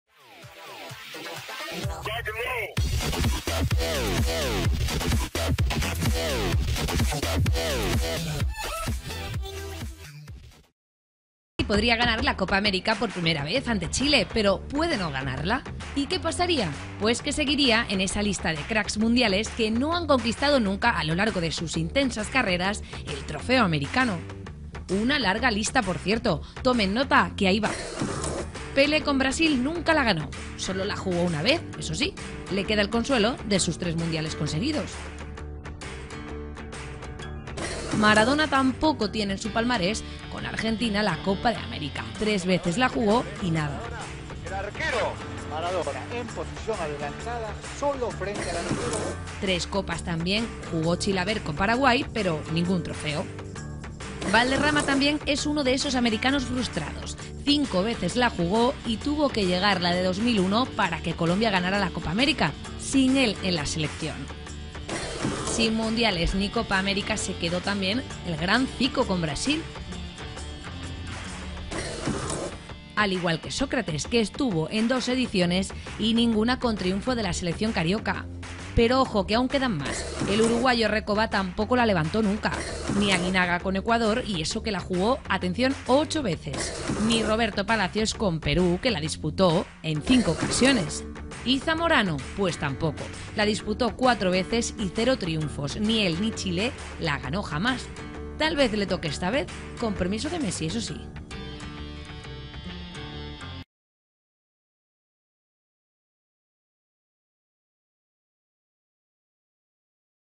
Diez craks del fútbol que nunca han ganado la Copa América ● FINAL COPA AMERICA 2015
● Ten craks football who have never won the Copa America ●

● Dix craks le football qui n'a jamais remporté la Copa America ●